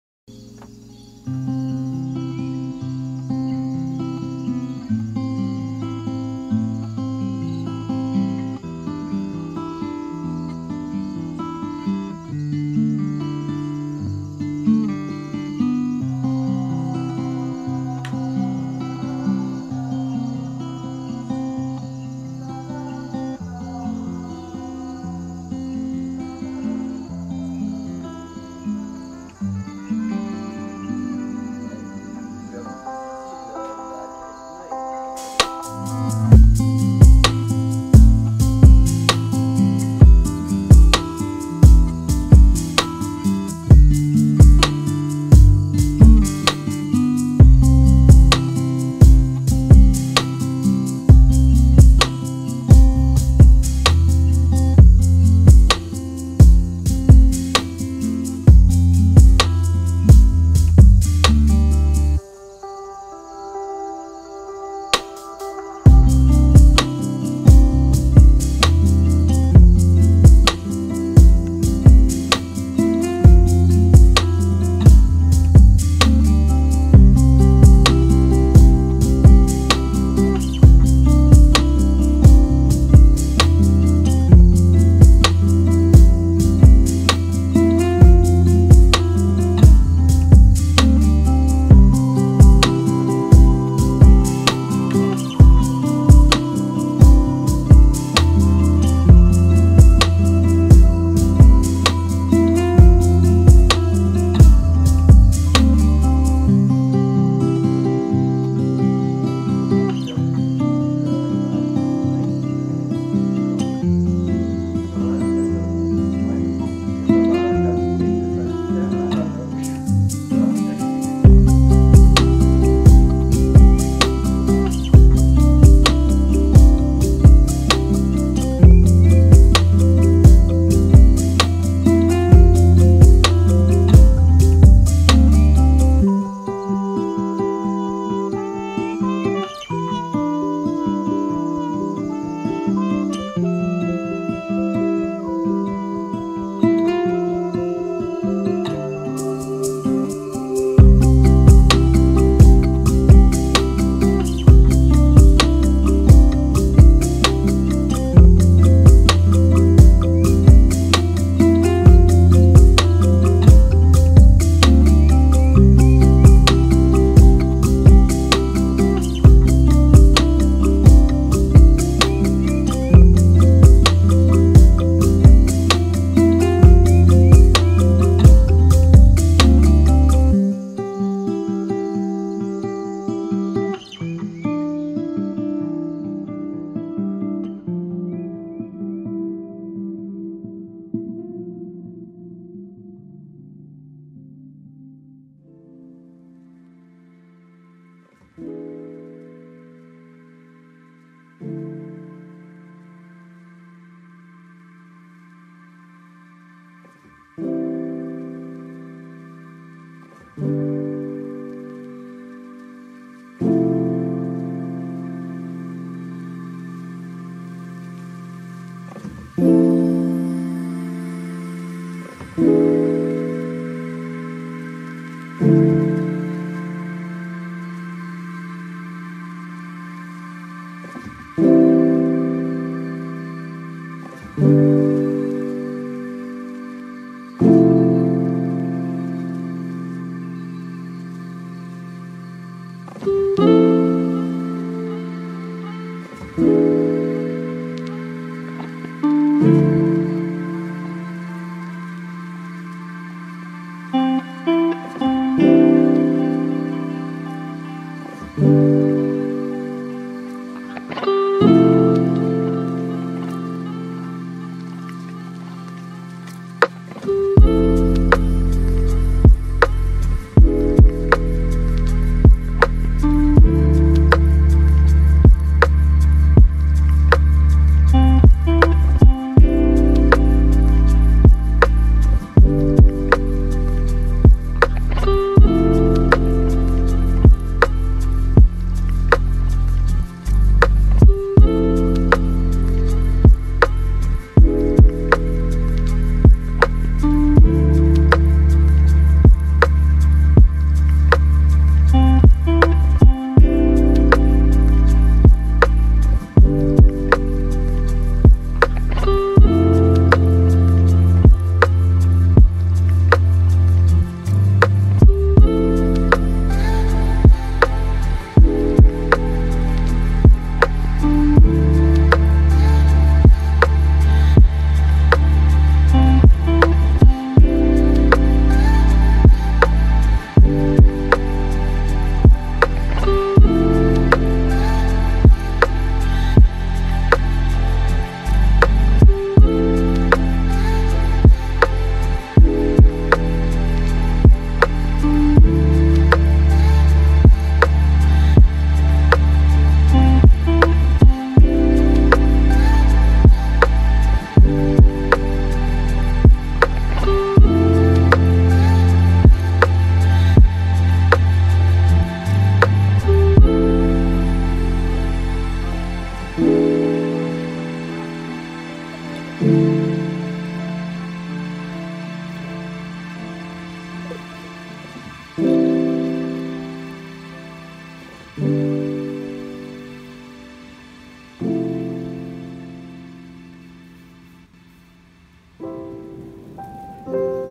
aesthetic mushroom jar 🧚‍♀️
fairy house🧚‍♂️🧚( i used air dry clay )

share your art on insta inspired by the channel → # artewithane